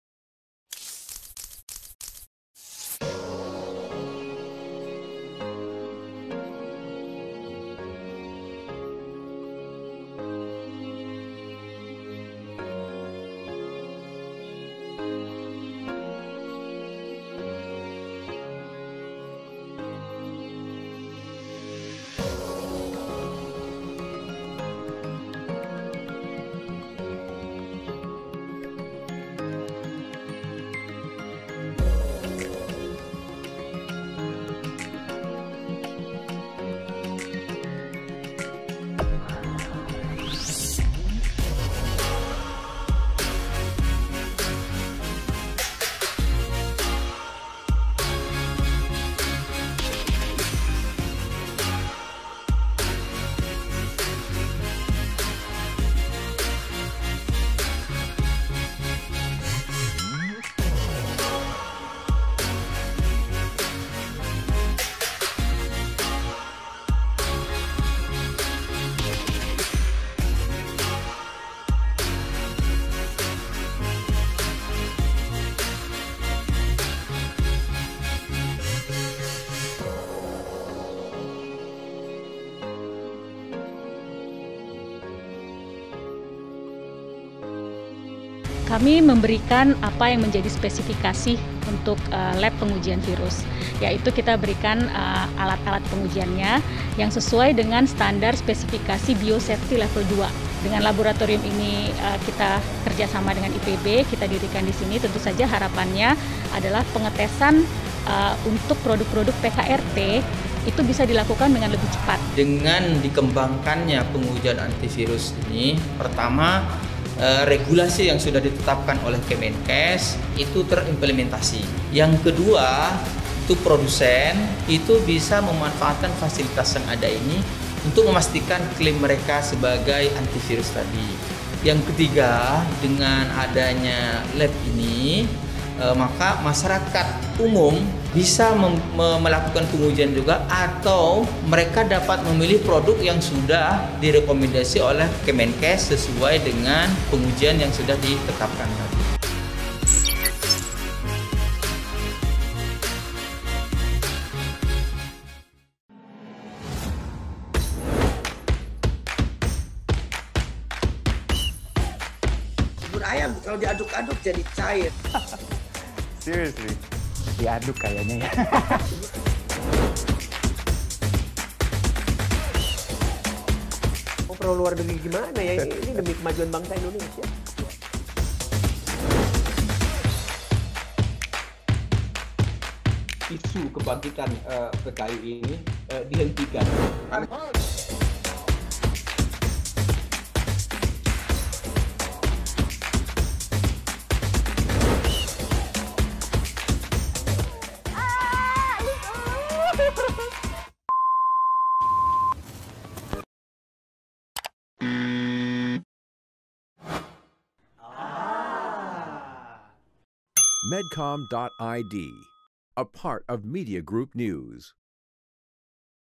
Pertama di Indonesia, IPB Kenalkan Laboratorium Uji Anti Virus
Memiliki produk Perbekalan Kesehatan Rumah Tangga (PKRT) yang teruji dan terverifikasi tentu penting bagi masyarakat. Maka dari itu telah hadir Laboratorium Jasa Pengujian dan sertifikasi pertama di Indonesia untuk produk Perbekalan Kesehatan Rumah Tangga di Institut Pertanian Bogor. Selain untuk virus, Laboratorium Mikrobiologi ini juga digunakan untuk pengujian bakteri dan fungi. Laboratorium ini telah memiliki tingkat keamanan biologi atau biosafety level 2 (BSL-2) dan tengah disiapkan menuju BSL-3.

Tak hanya menjadi tempat pengujian mikrobiologi, Unit Laboratorium Jasa Pengujian dan Sertifikasi IPB University kini juga berfungsi sebagai lembaga pemeriksa halal. Pasalnya, saat ini label halal menjadi salah satu kriteria penting selain kualitas dari suatu produk.